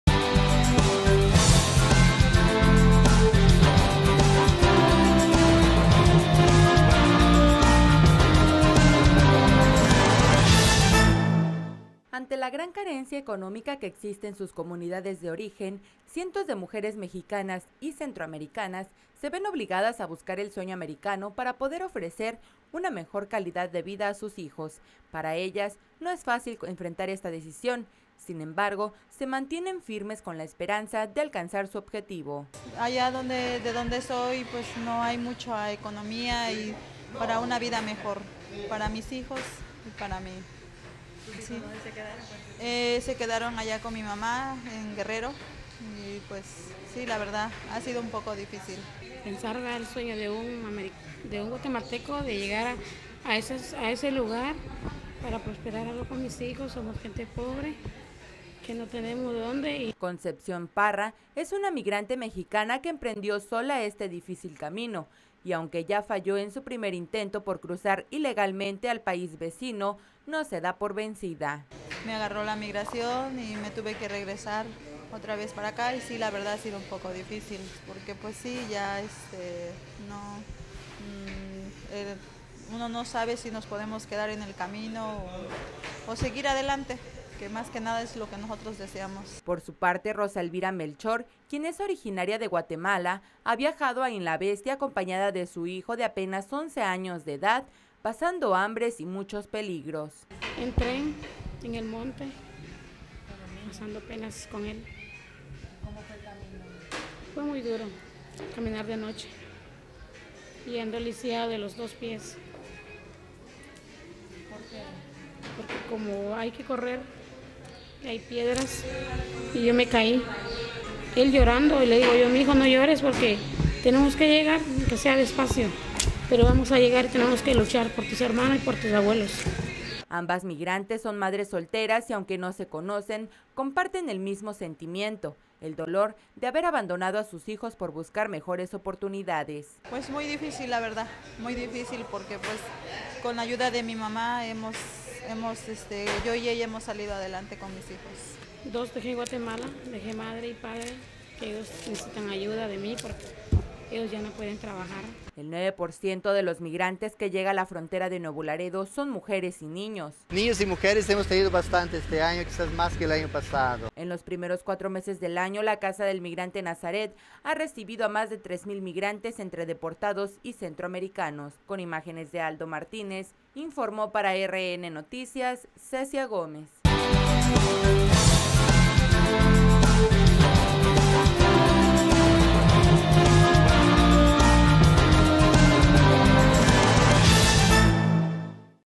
Madres solteras migran para mejorar calidad de vida de sus hijos
Ante la gran carencia económica que existe en sus comunidades de origen, cientos de mujeres mexicanas y centroamericanas se ven obligadas a buscar el "sueño americano" para poder ofrecer una mejor calidad de vida a sus hijos.
Para ellas no es fácil enfrentar esa decisión, sin embrago se mantienen firmes con la esperanza de alcanzar su objetivo.
Concepción Parra Hernández, una mujer migrante, comentó "Allá de donde soy no hay mucha economía y para una vida mejor para mis hijos y para mí, se quedaron allá con mi mamá en Guerrero y pues si la verdad ha sido un poco difícil"
Por su parte Rosa Elvira Melchor Ramos, agregó "El sueño de un Guatemalteco para llegar a ese lugar prosperar algo con mis hijos somos gente pobre que no tenemos de donde"
Concepción Parra, es una migrante mexicana que emprendió sola este difícil camino y aunque ya falló en su primer intento por cruzar ilegalmente al país vecino, no se da por vencida.
"Me agarro la migración  y me tuve que regresar otra vez para acá y si la verdad ha sido un poco difícil porque pues ya uno no sabe si nos podemos quedar en el camino o seguir adelante que más que nada es lo que nosotros deseamos" señaló Concepción.
Por su parte Rosa Elvira Melchor, quien es originaria de Guatemala ha viajado en "la bestia" en compañía de su pequeño hijo de apenas 11 años de edad pasando hambres y muchos peligros.